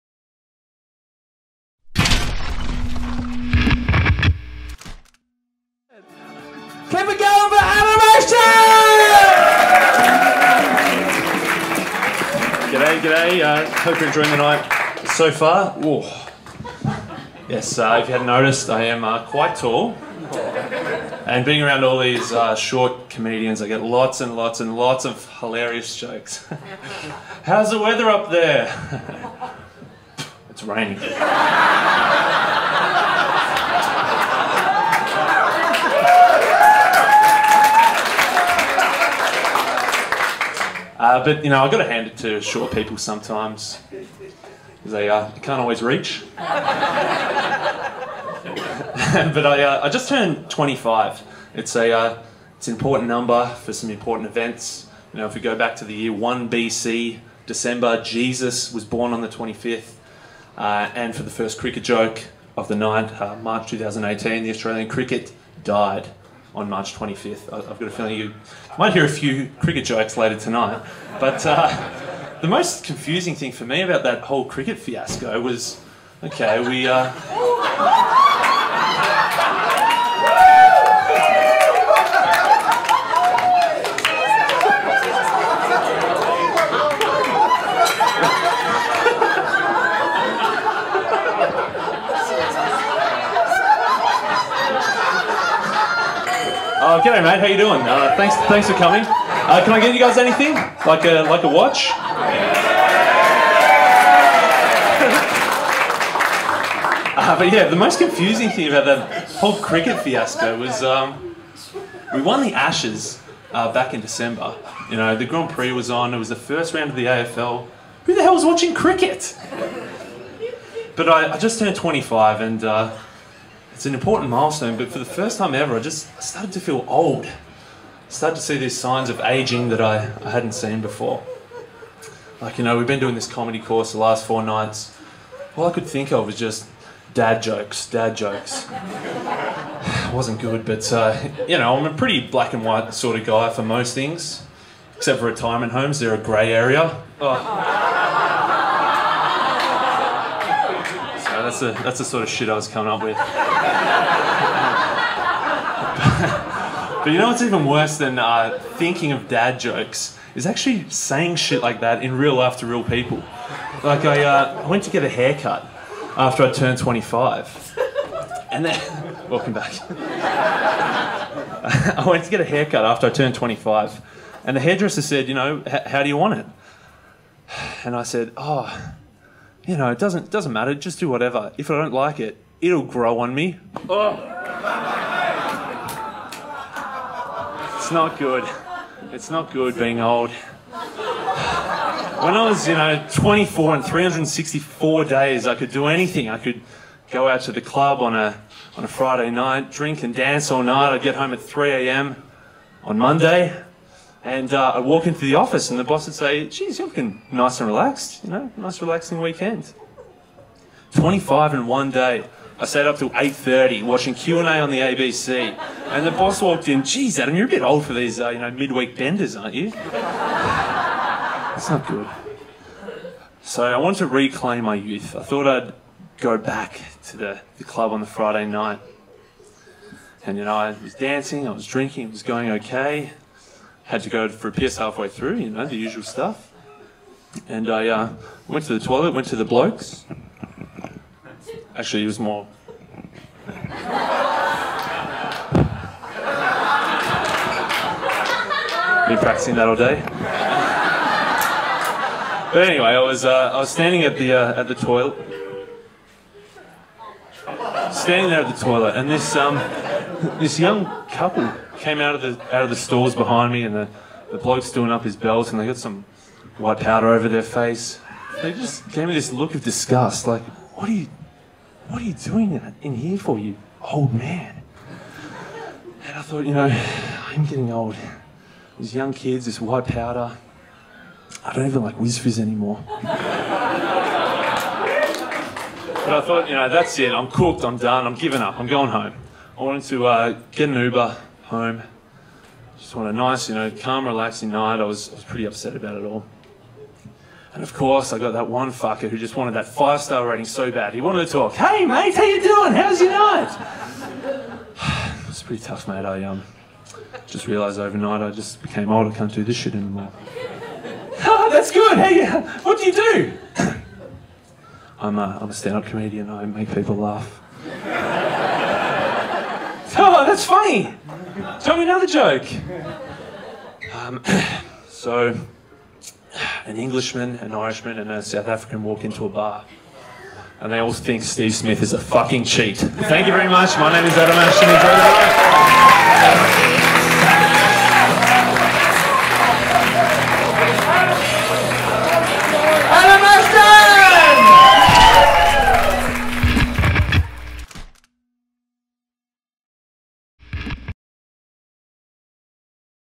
Adam Ashton - School of Hard Knock Knocks - Learn stand-up comedy in Melbourne
Watch Adam Ashton perform his debut stand-up comedy set at the School of Hard Knock Knocks comedy course graduation in Melbourne.

Want to try stand-up comedy?